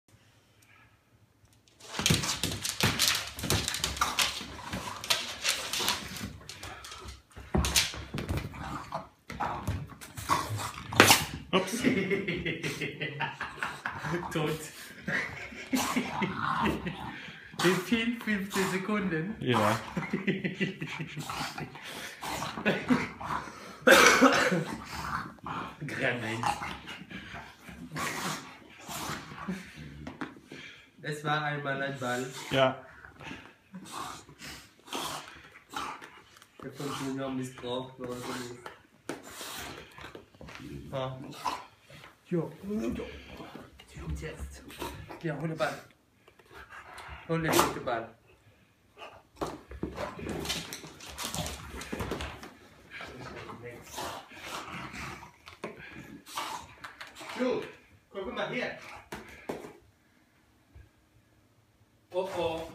Ball invasion - Junior loves to destroy balls. ;)
Junior loves to play and to destroy balls. We had a ball invasion in our home and Junior saved us from the aggressive invaders.

Junior liebe es mit Bällen zu spielen und auch diese zu zerstören. Wir hatten zu Hause eine feindliche Ballinvasion, aber Junior hat uns gerettet.

Junior is a french bulldoge. Follow him and his family.
Junior ist eine französische Bulldoge. Folge ihm und seiner Familie